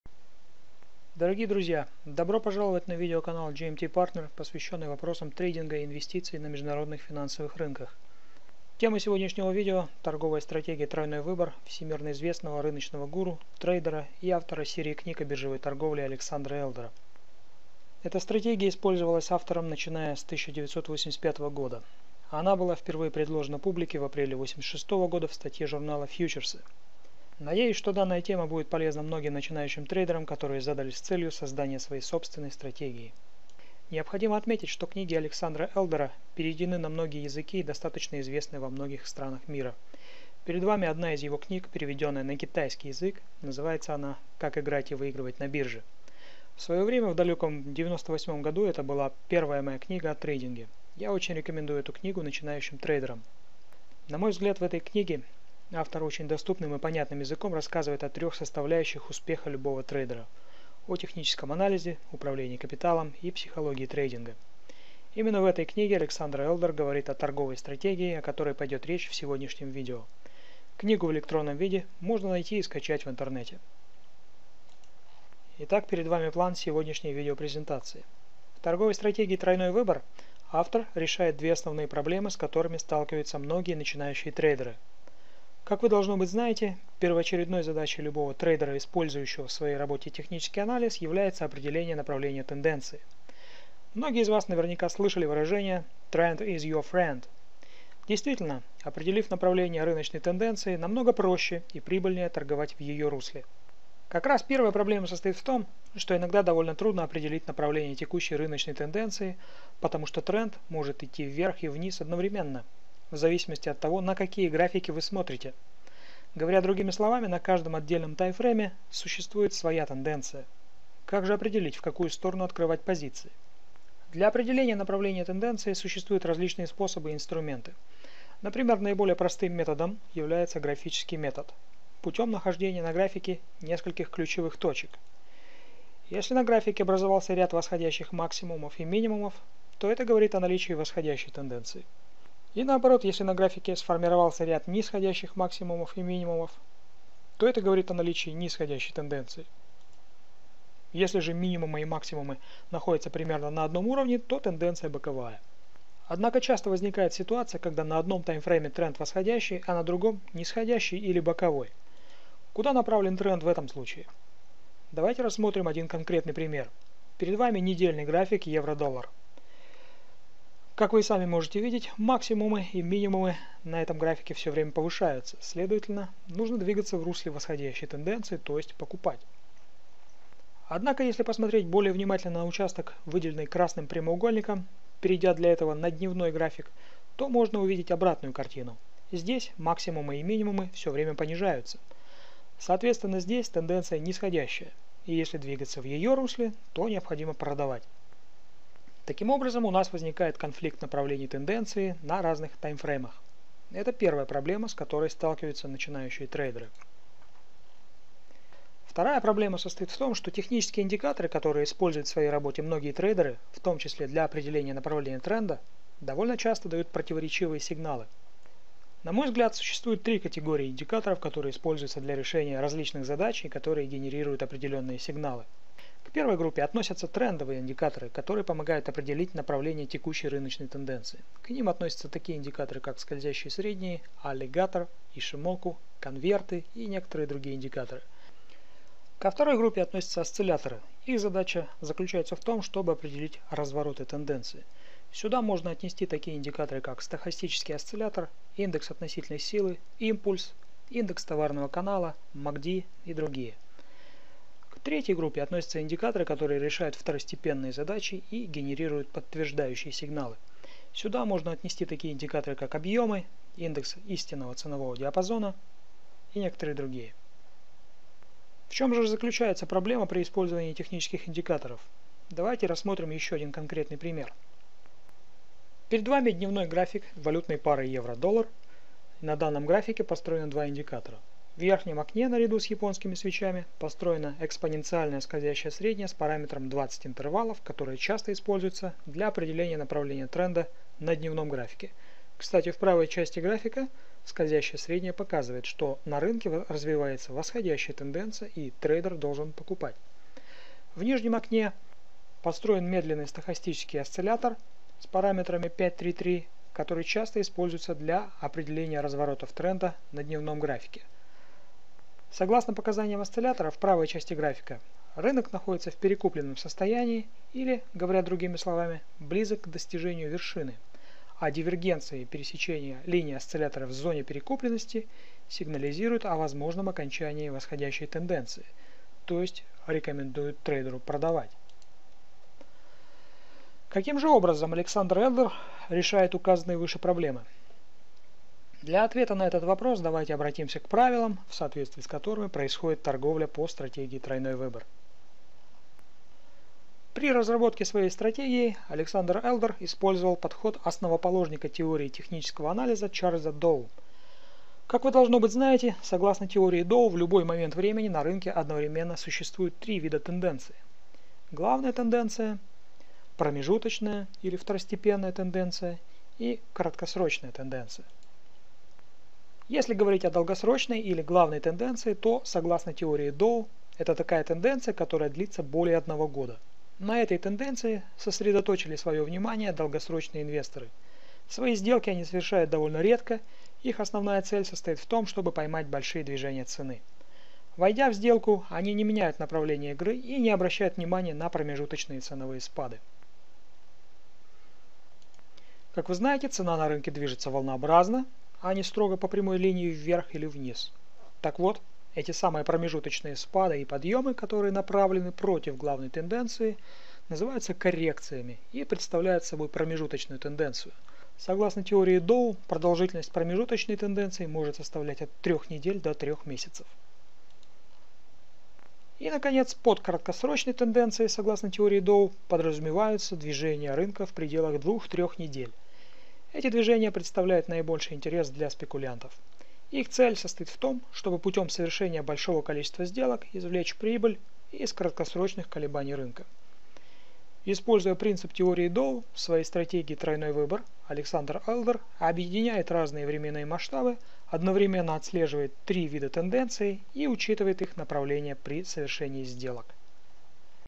Стратегия А Элдера Три Экрана Часть 1(6)
В данном видео идет речь о торговой стратегии Александра Элдера "Тройной выбор (Три экрана)". Часть 1: Проблемы начинающих трейдеров и концепция стратегии.